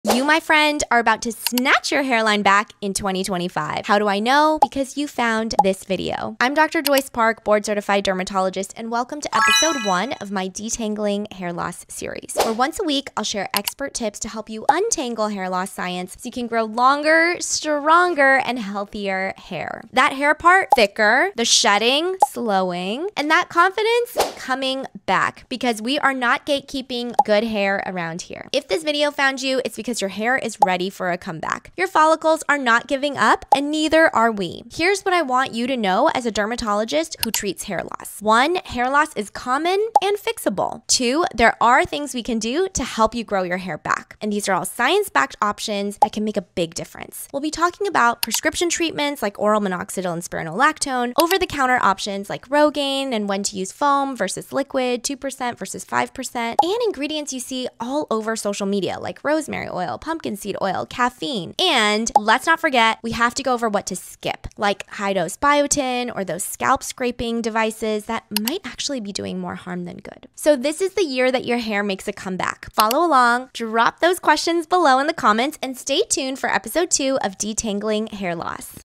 Detangling Hair Loss | Episode 1: Your Hair Comeback Starts Here
So many myths, so much confusion. It’s time to untangle the truth about hair loss. That’s why I’m so excited to launch my new series Detangling Hair Loss.

Each week, I’ll break down the science of hair loss and share the treatments that actually work—no fluff, no gatekeeping.

I can’t wait to share this journey with you!